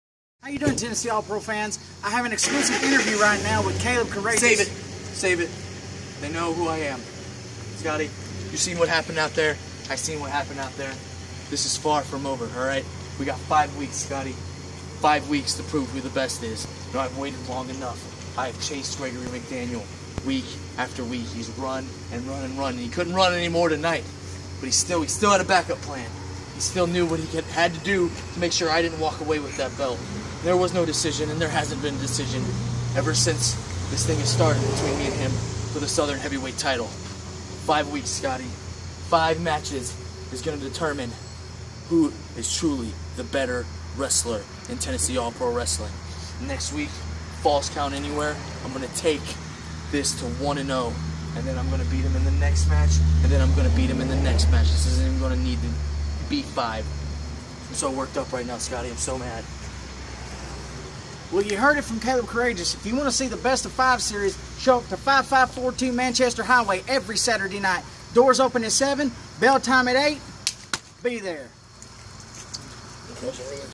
TAPW Exclusive Interview Caleb Courageous Sept 26
Exclusive interview with Caleb Courageous after the shoe September 26, 2015